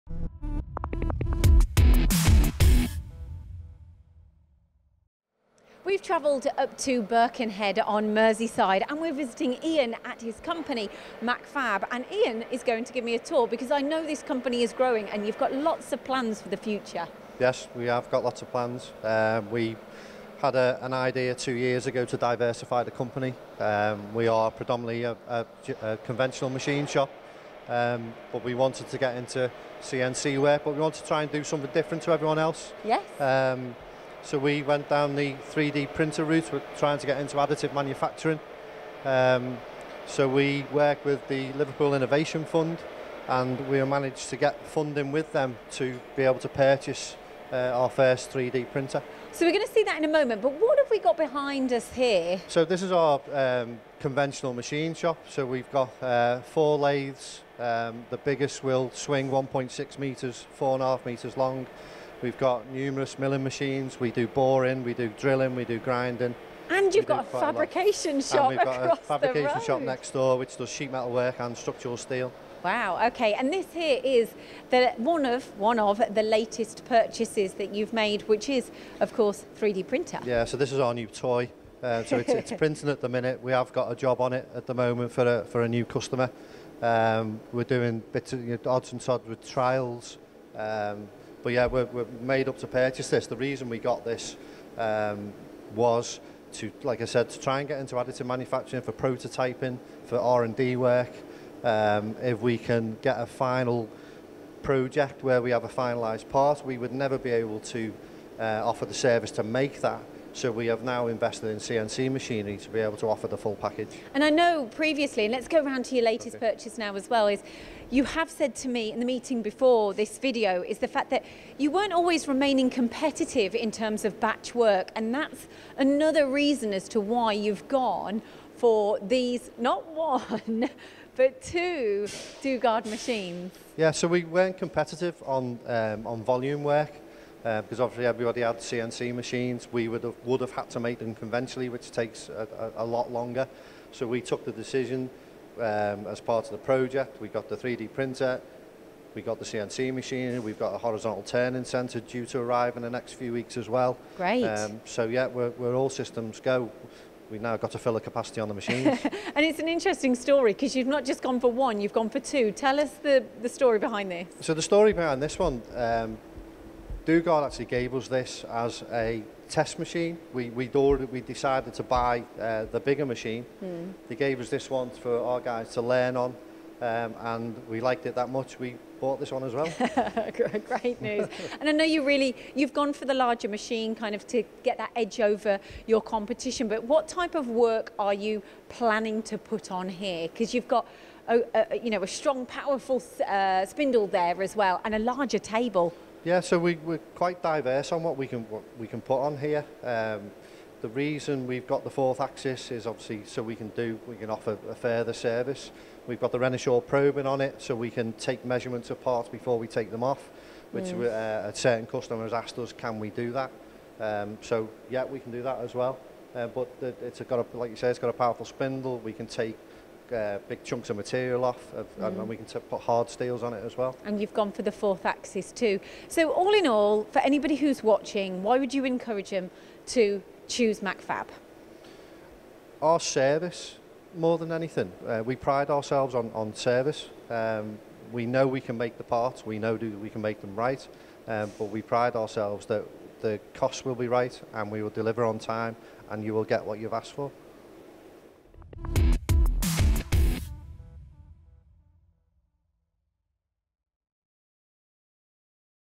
Looking for a Fab subcontractor? How about Machfab...
Lyndsey traveled up to Liverpool to meet with Machfab owner Ian Hazlehurst. His company has just invested in new technologies, interestingly two differing types, subtractive and additive. They take a tour of the factory and we capture it all on camera.